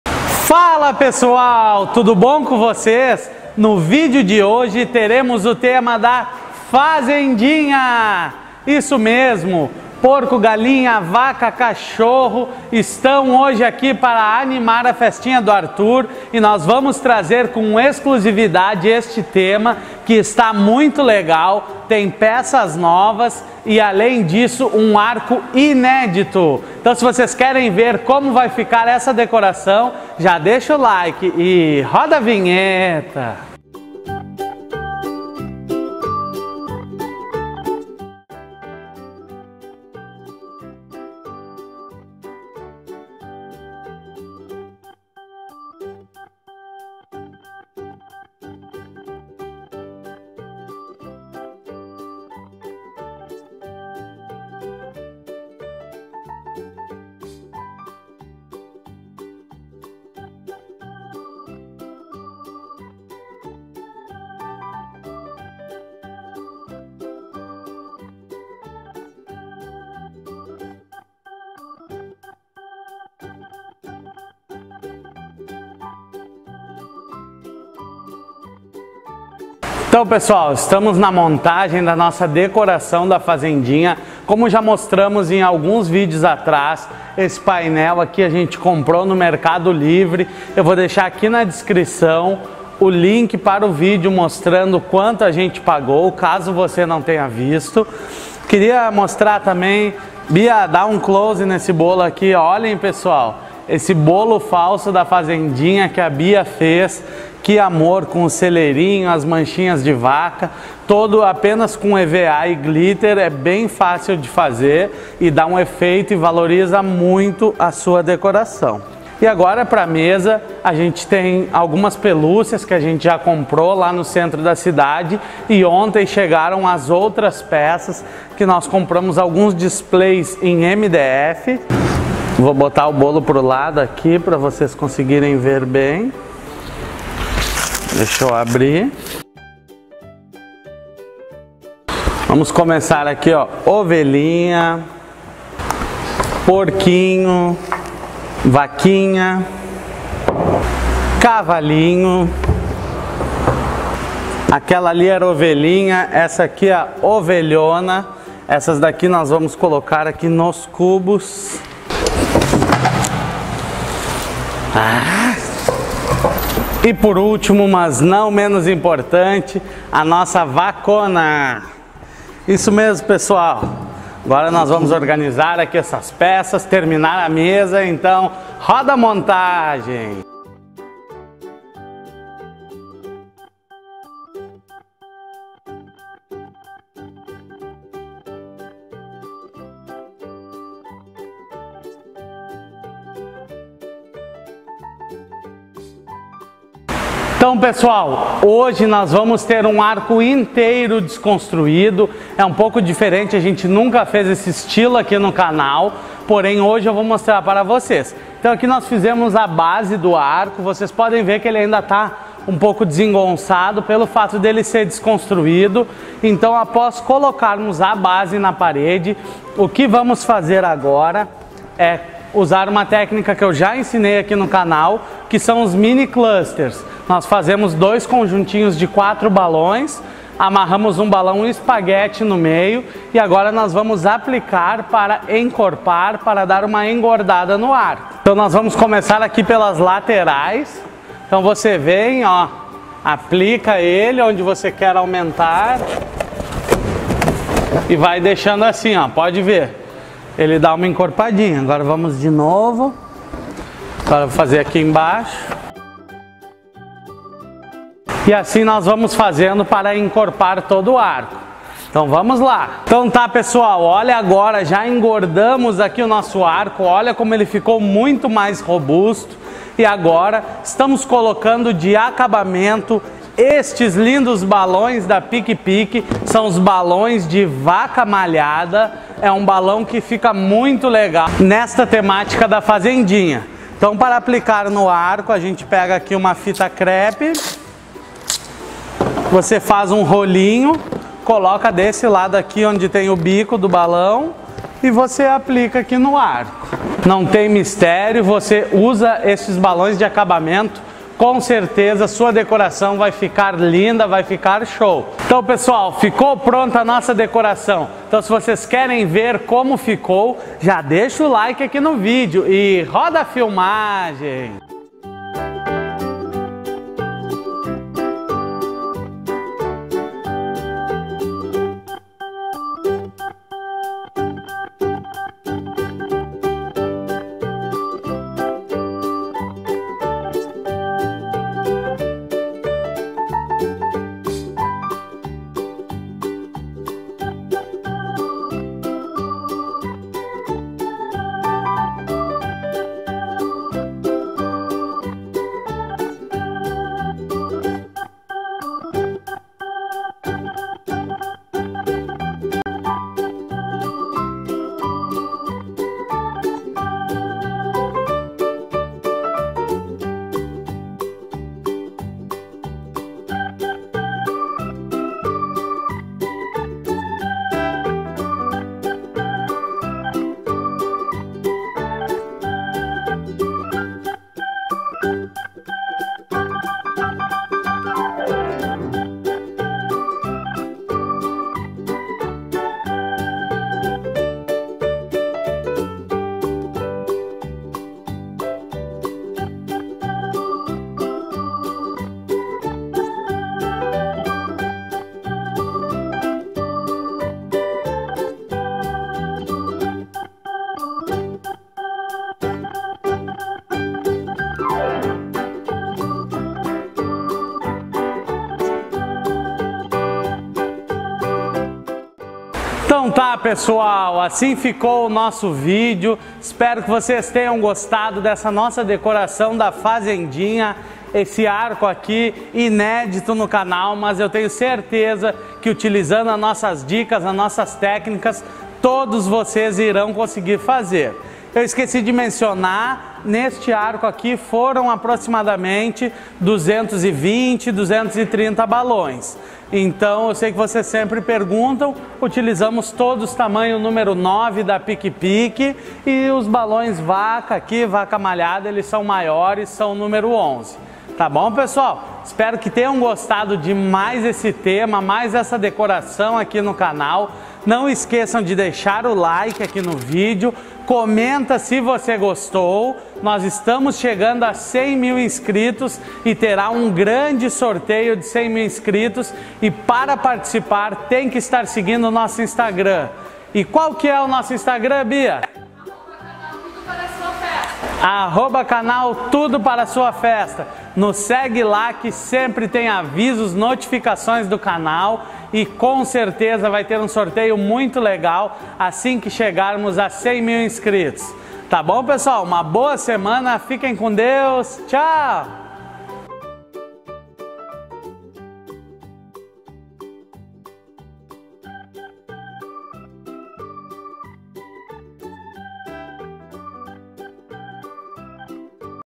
Decoração FAZENDINHA - Arco INÉDITO no Canal!
Fala pessoal, tudo bom com vocês? No vídeo de hoje mostramos a decoração da Fazendinha. Um tema para meninos e meninas, que encanta muito as mamães e papais que não querem um tema com personagens definidos! Espero que gostem!

Siga o nosso Instagram @CanalTudoParaSuaFesta

ME ENVIE PRESENTES 🎁 OU MERCADORIAS 📦 PARA DIVULGAÇÃO !
Caixa Postal: 13016
CEP: 91010971